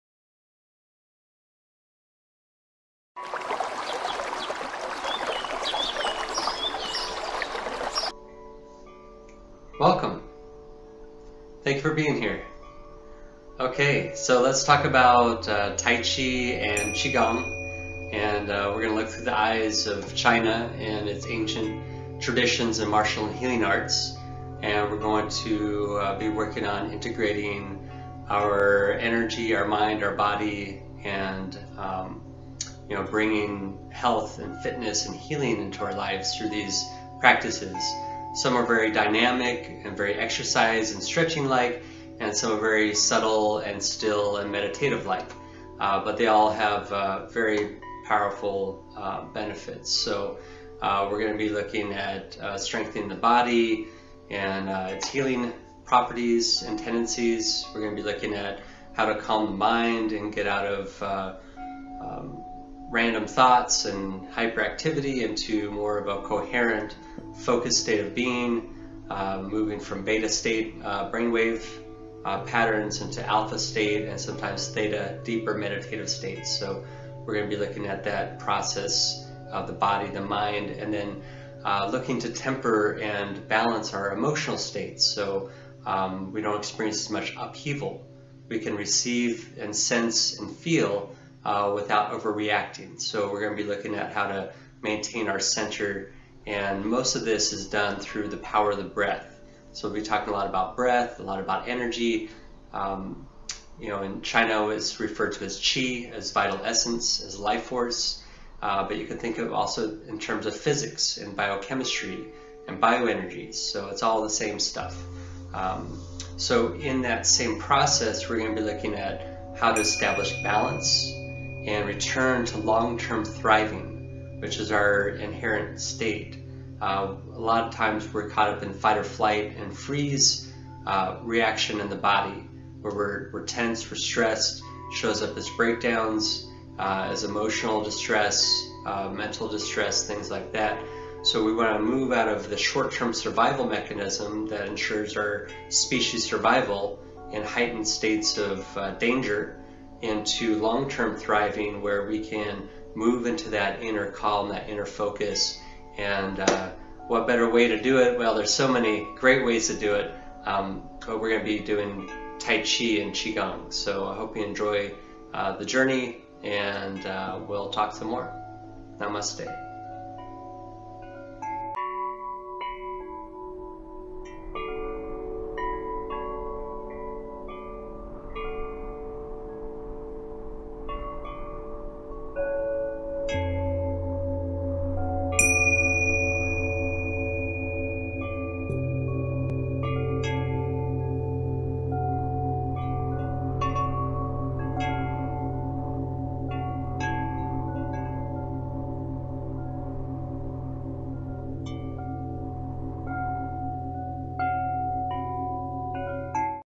Why Having an Energy, Mind, Body Practice Changes Lives: Welcome to Tai Chi & Qi Gong With On Path
Invite health, healing, and happiness into your life with these powerful practices and traditions of Tai Chi and Qi Gong with Seifu Andy at On-Path.com.

Learn to relieve and prevent stress reactivity and return to your natural state of Long-Term Thriving.

Learn to cultivate chi (vital essence), quiet your mind, balance your emotions, and relax and strengthen your body.

Why Practice? There are so many great and obvious reasons, yet there are so  many that are subtle and take time to grow, often creating a profound shift in our quality of life and experience.

It is what we do everyday or routinely that has the greatest effect on our life experience. This helps to shape the moment of NOW, but it also impacts our future experiences, choices, and opportunities.

What we repeat, we become. Add a sprinkle of Tai Chi, Qi Gong, Meditation, and Energy Awareness into your life and see for yourself why millions of people participate in energy, mind, and body practices.

Move Your Body, Free Your Mind, and Energize Your Spirit
On-Path.com

Peace, Love, and Chi-Up

Seifu Andy